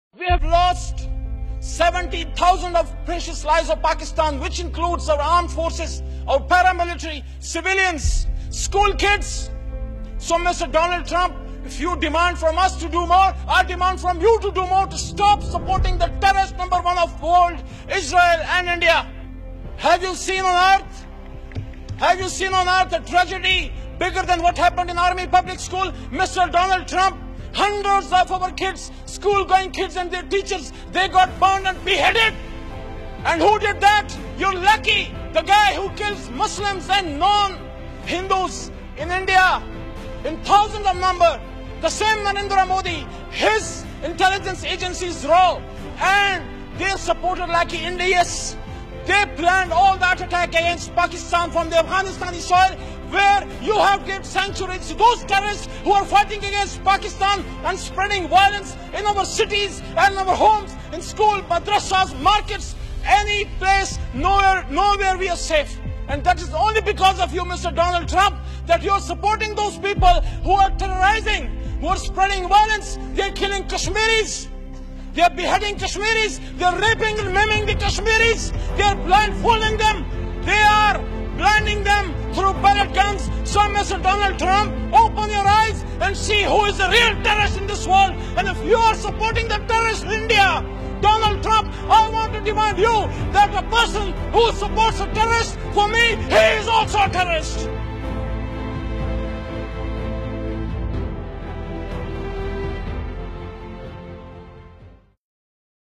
Request to the world , plz stop terrorists
A speech in Pakistani National Assembly , Video from Malik Zaman